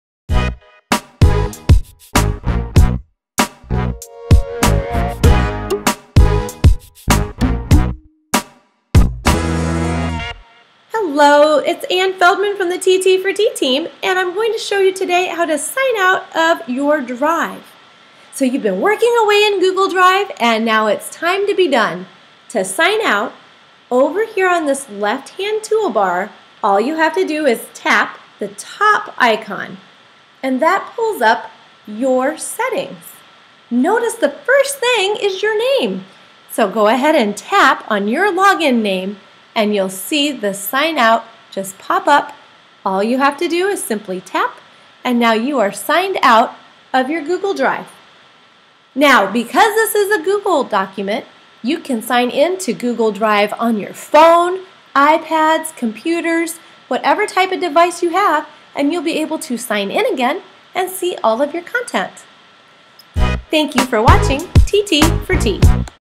Google Drive iPad - Signing Out
This video discuss how to sign out of Google Drive on the iPad